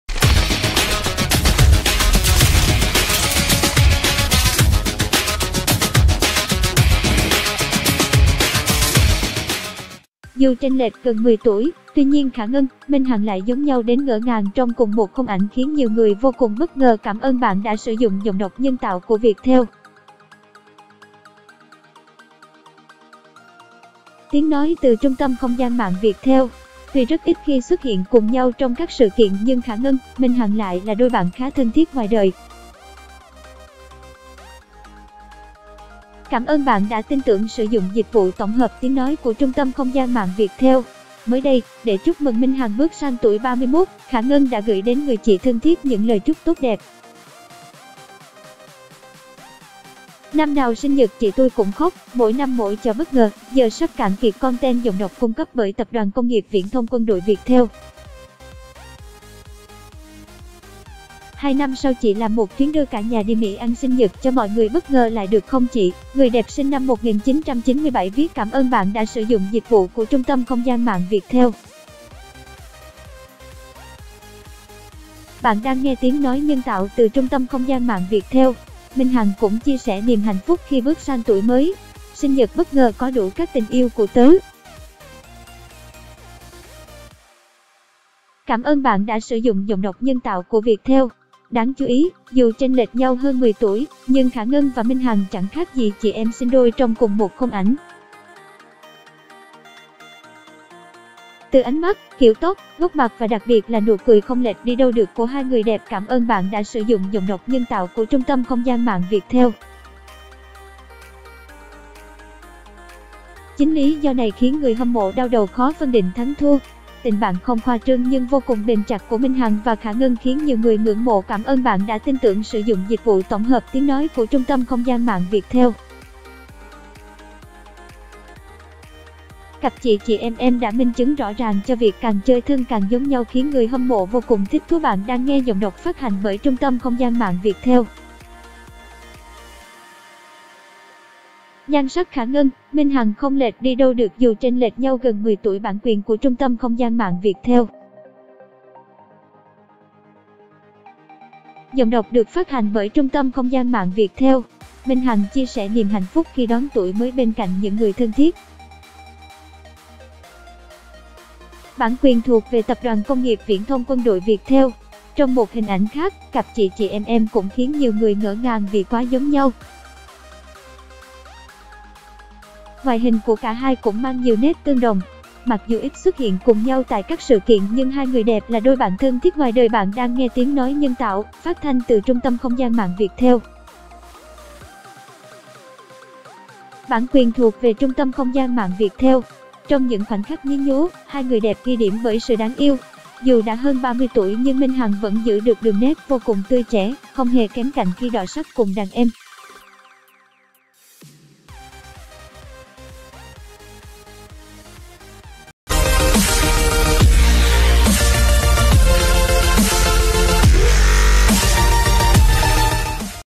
Tin Đc Ko - Khả Ngân xúc động trong sinh nhật Minh Hằng, nhưng nhan sắc giống nhau đến ngỡ ngàng mớ
Dù chênh lệch gần 10 tuổi, tuy nhiên Khả Ngân – Minh Hằng lại giống nhau đến ngỡ ngàng trong cùng một khung ảnh khiến nhiều người vô cùng bất ngờ. Tuy rất ít khi xuất hiện cùng nhau trong các sự kiện nhưng Khả Ngân – Minh Hằng lại là đôi bạn khá thân thiết ngoài đời. Mới đây, để chúc mừng Minh Hằng bước sang tuổi 31, Khả Ngân đã gửi đến người chị thân thiết những lời chúc tốt đẹp. “Năm nào sinh nhật chị tui cũng khóc, mỗi năm mỗi trò bất ngờ, giờ sắp cạn kiệt content. Hay năm sau chị làm một chuyến đưa cả nhà đi Mỹ ăn sinh nhật cho mọi người bất ngờ lại được không chị”, người đẹp sinh năm 1997 viết. Minh Hằng cũng chia sẻ niềm hạnh phúc khi bước sang tuổi mới: “Sinh nhật bất ngờ có đủ các tình yêu của tớ!”. Đáng chú ý, dù chênh lệch nhau hơn 10 tuổi, nhưng Khả Ngân và Minh Hằng chẳng khác gì “chị em sinh đôi” trong cùng một khung ảnh. Từ ánh mắt, kiểu tóc, góc mặt và đặc biệt là nụ cười không lệch đi đâu được của 2 người đẹp. Chính lí do này khiến người hâm mộ đau đầu khó phân định thắng thua. Tình bạn không khoa trương nhưng vô cùng bền chặt của Minh Hằng và Khả Ngân khiến nhiều người ngưỡng mộ. Cặp “chị chị em em” đã minh chứng rõ ràng cho việc càng chơi thân càng giống nhau khiến người hâm mộ vô cùng thích thú.Nhan sắc Khả Ngân – Minh Hằng không lệch đi đâu được dù chênh lệch nhau gần 10 tuổi.Minh Hằng chia sẻ niềm hạnh phúc khi đón tuổi mới bên cạnh những người thân thiết.Trong một hình ảnh khác, cặp “chị chị em em” cũng khiến nhiều người ngỡ ngàng vì quá giống nhau.Ngoại hình của cả hai cũng mang nhiều nét tương đồng.Mặc dù ít xuất hiện cùng nhau tại các sự kiện nhưng 2 người đẹp là đôi bạn thân thiết ngoài đời.Trong những khoảnh khắc nhí nhố, 2 người đẹp ghi điểm bởi sự đáng yêu. Dù đã hơn 30 tuổi nhưng Minh Hằng vẫn giữ được đường nét vô cùng tươi trẻ, không hề kém cạnh khi đọ sắc cùng đàn em.